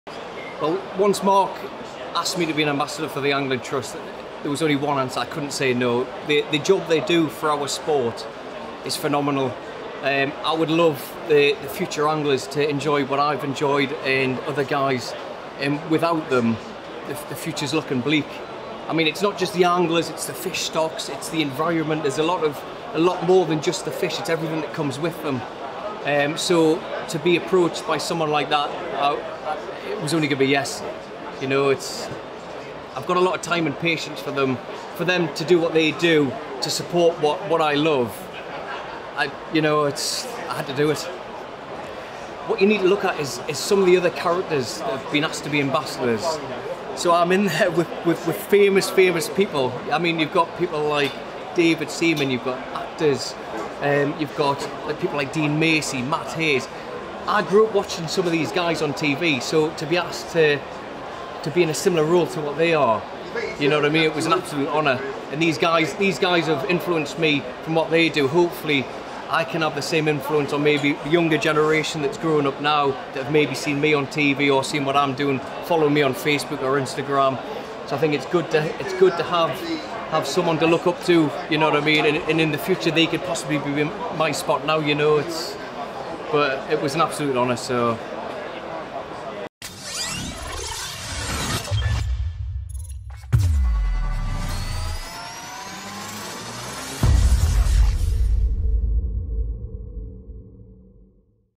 James Stokoe, Angling Trust Ambassador
TV's The Big Fish Off winner James Stokoe talks to Tacklebox TV about his role as Ambassador with the Angling Trust